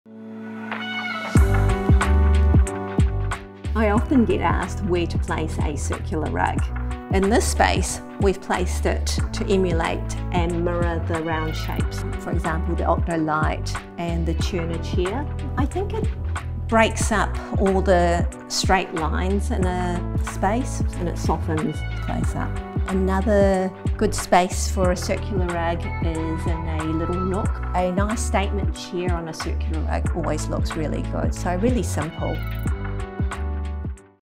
The Designer Series 1 - Lily Ng | Placing a circular rug
Introducing the first instalment of our Designer Series. Our very own Designer Lily Ng explores the beautiful 'Under Pohutukawa' beach house in Piha.

A circular rug can be a stunning statement piece for your home.

Lily shows us some of the best ways to place a circular rug to maximise its impact.